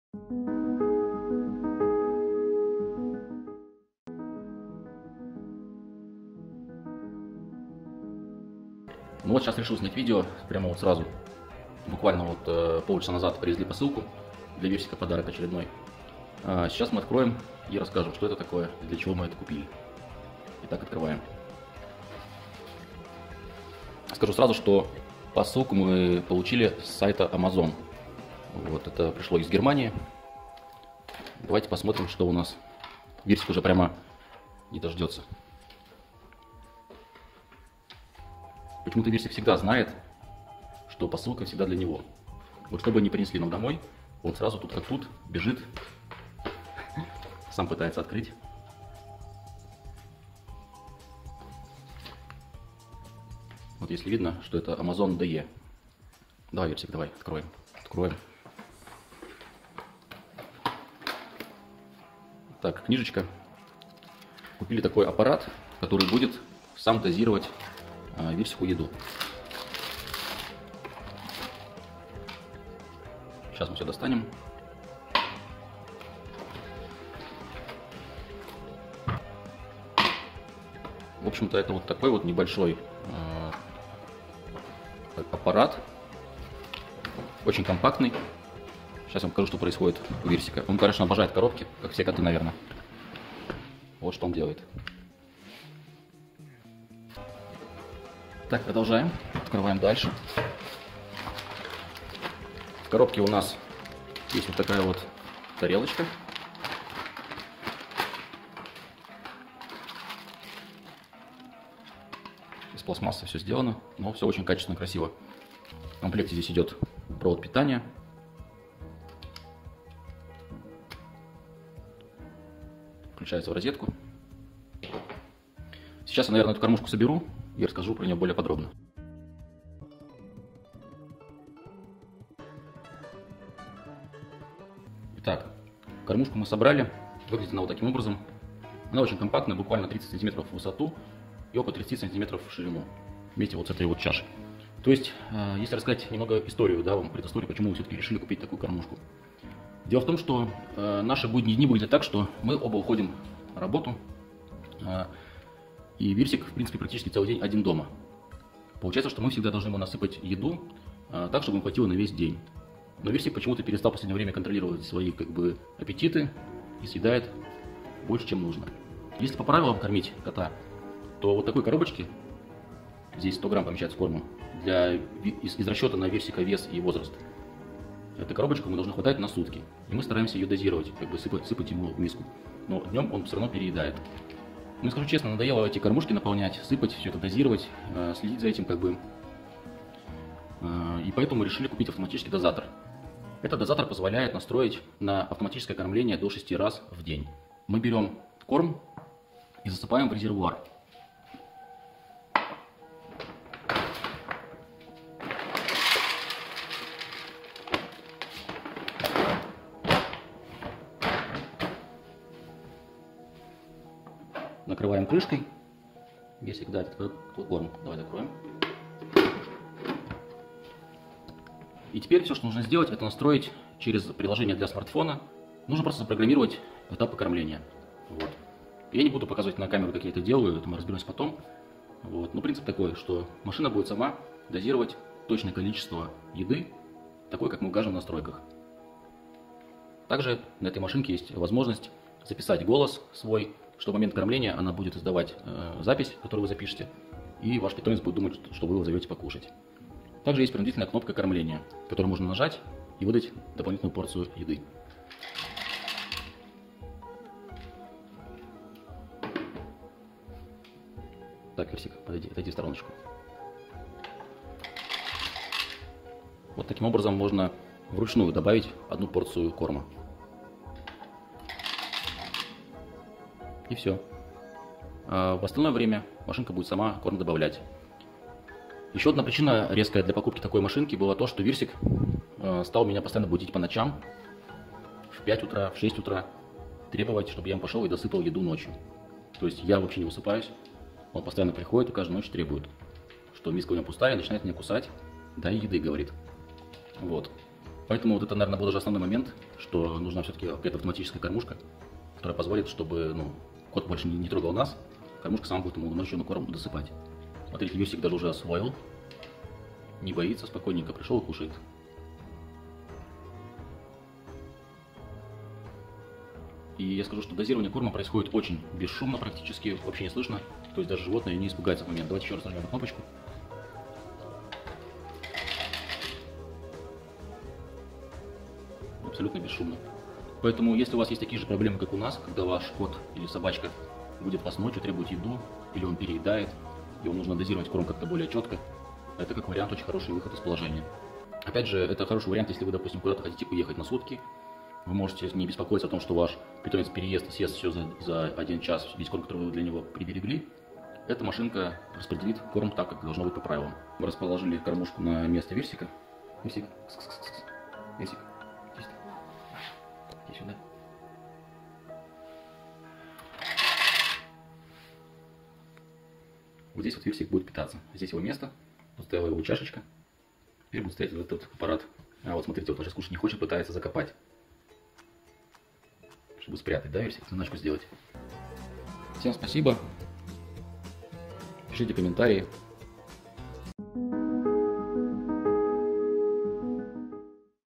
Умная автоматическая кормушка с диктофоном, для кошек и собак
Эта автоматическая кормушка для домашних животных очень поможет вам.
Вы можете использовать приложение, чтобы настроить время и порции еды.